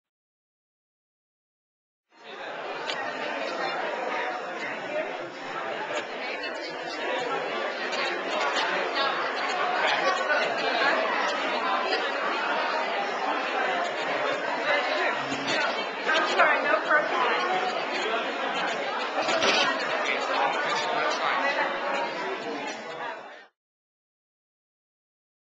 Nobel Laureate Orhan Pamuk in Houston
After a terrific reading and interview by Chitra Divakaruni, Orhan Pamuk signed books for his audience.